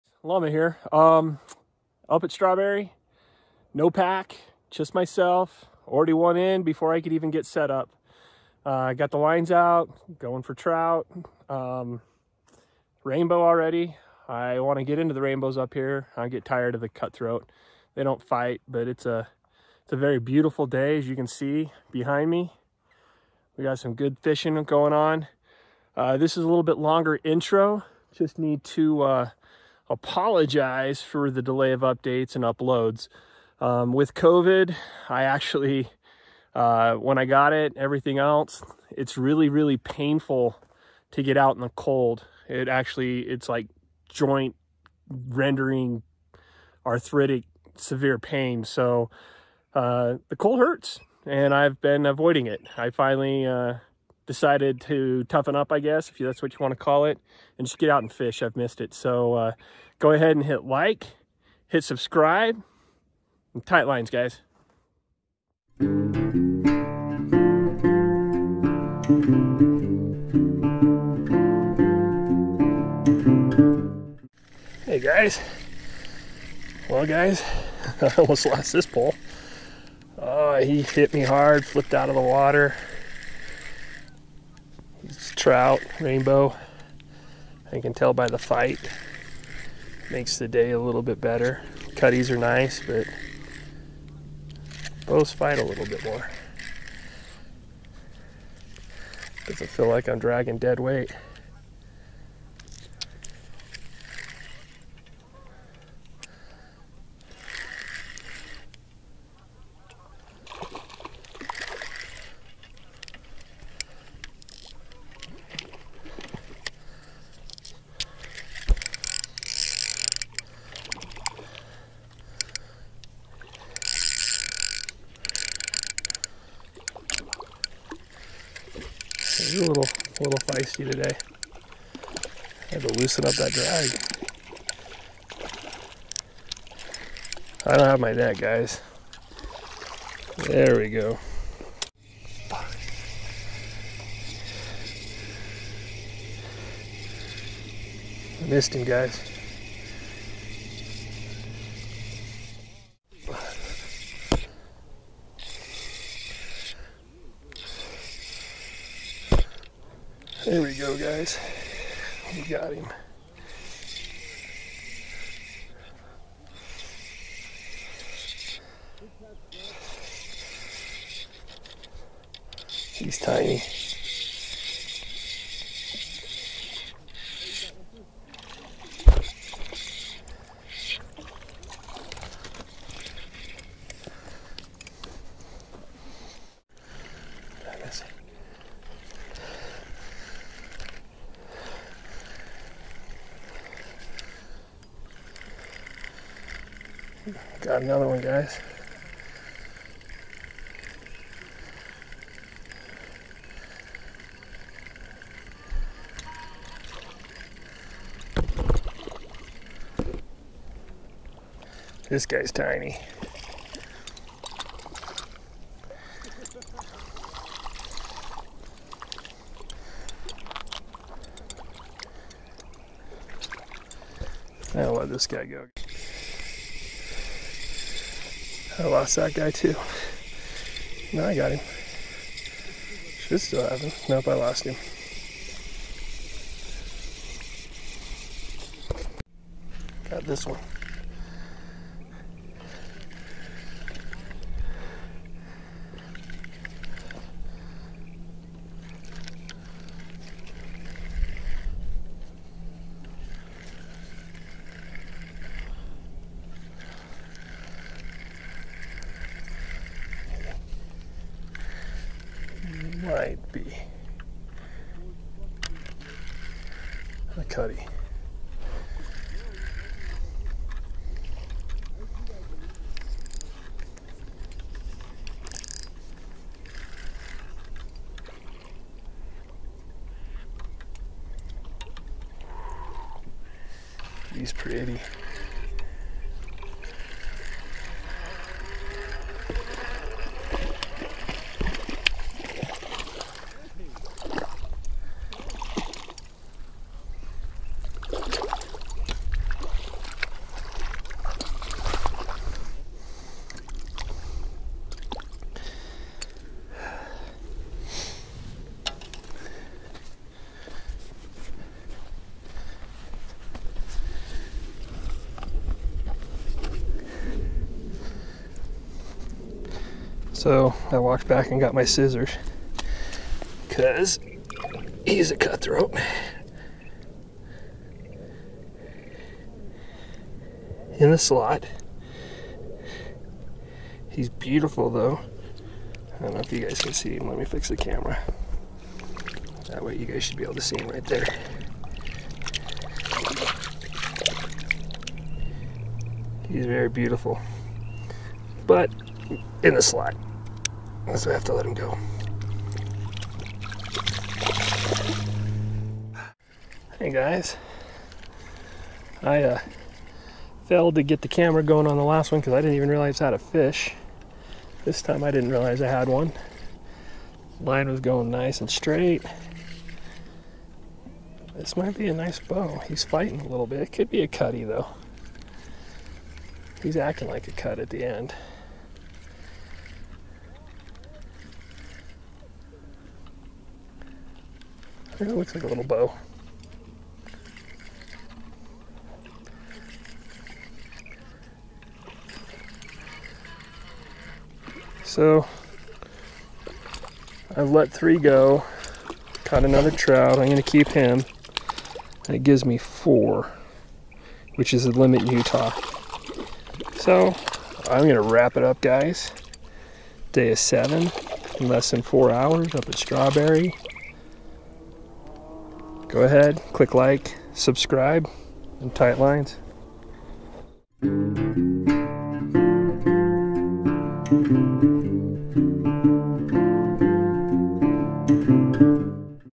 The Llama Limits out with Rainbow Trout at Strawberry Reservoir in a couple of hours.
Greetings Pack!

I decided to head up to Strawberry and see if I could get into the Rainbow Trout instead of the Cutthroats like last year.

I was not disappointed. With my first cast and not setting up, I had one on the line and let him go, finished prepping and recast.

They weren't hitting as hard as they normally do, just taking it and running and spitting out the minute they tasted metal - but it was fun when they did hit hard.

Put 4 on my stringer to bring home and smoke (Videos will be coming later for the smoke)

Tight Lines Guys!